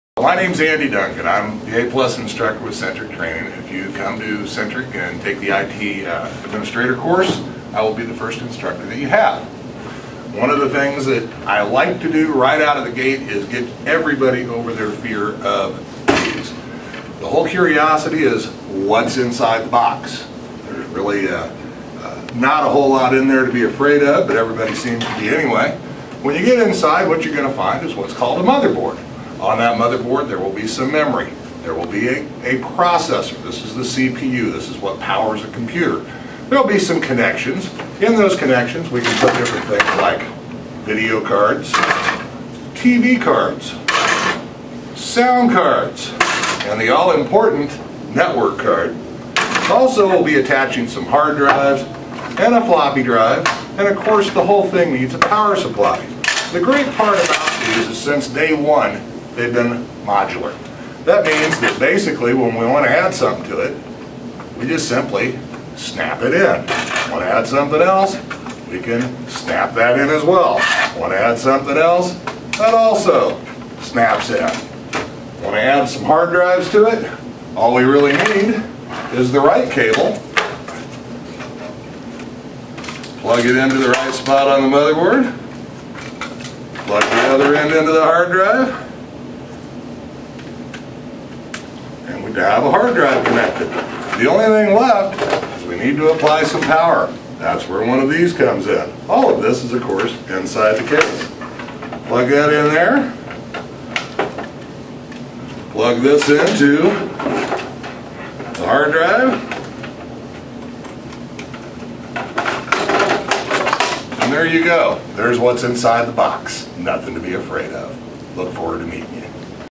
Andy Duncan: A+ Certification Instructor
The first instructor a student meets on the I.T. Administrator track. Andy will teach you the basics for your A+ certification no matter what knowledge level you walk into his classroom with on the first day.